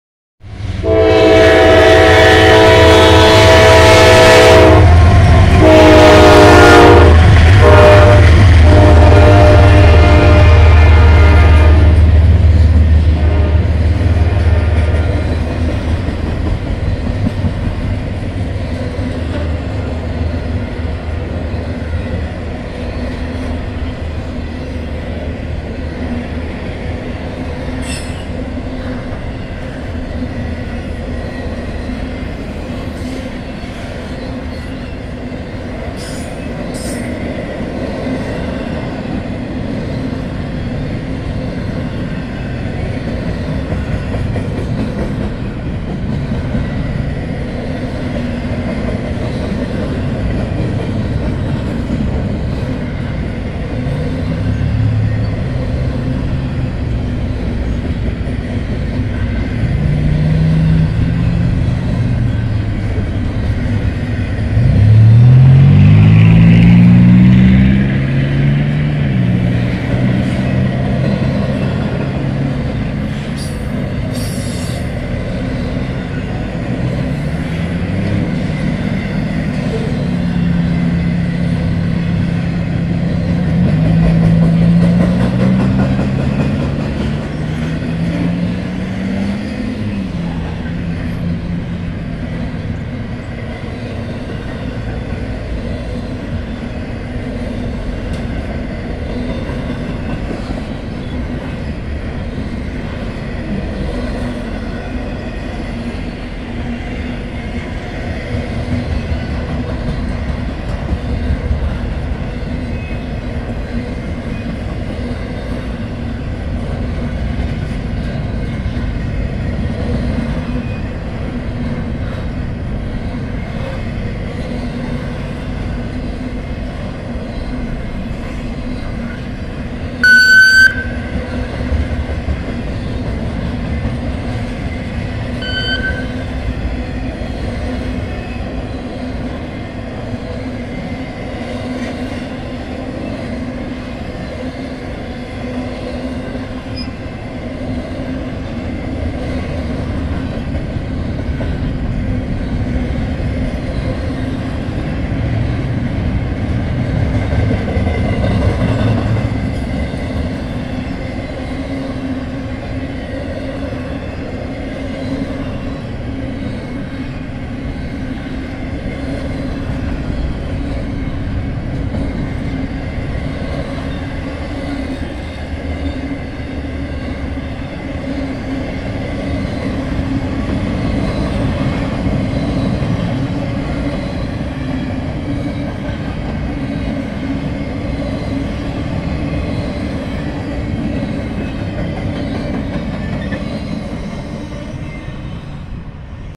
Southbound NS Intermodal 6-14-2023
The last train of the day happened on the way out of town. Just north of Burger Hut, this intermodal came flying through. this perfect ending encapsulates a trip to Alliance with the Alliance City board.